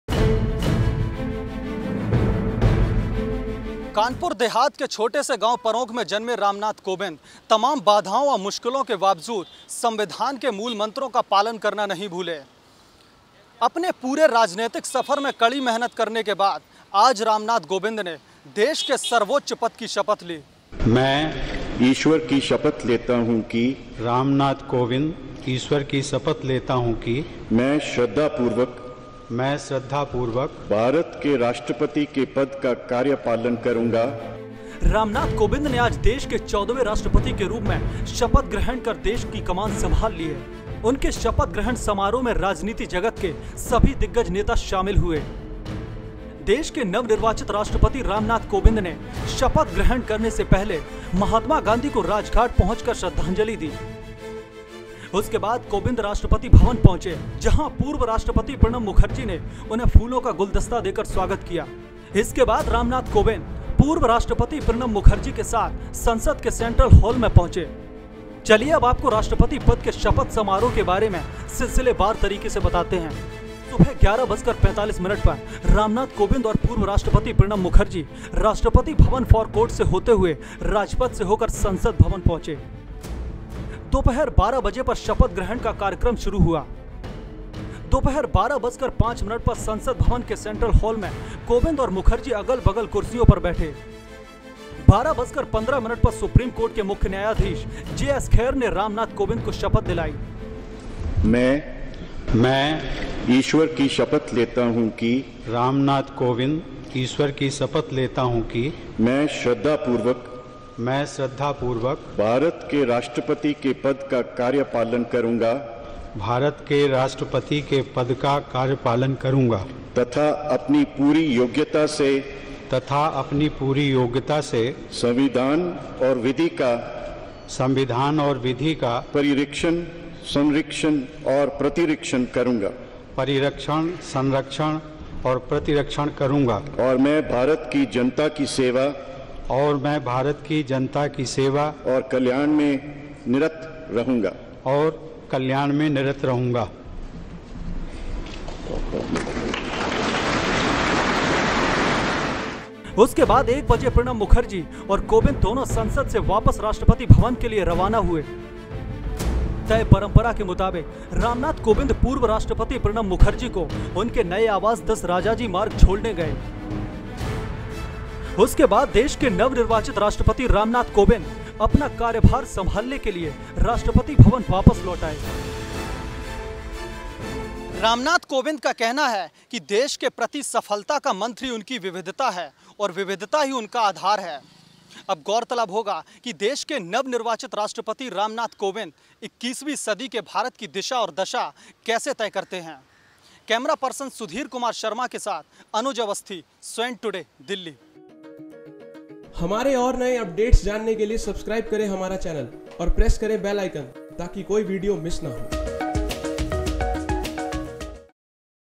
भारत के नए राष्ट्रपति रामनाथ कोविंद के शपथ समारोह की पूरी जानकारी
मंगलवार (25 जुलाई) को संसद के केंद्रीय कक्ष में सुप्रीम कोर्ट के मुख्य न्यायाधीश जेएस खेहर ने उन्हें पद की शपथ दिलायी। देश के 14वें राष्ट्रपति के तौर पर शपथ लेने के बाद रामनाथ कोविंद को गॉर्ड ऑफ ऑनर दिया गया।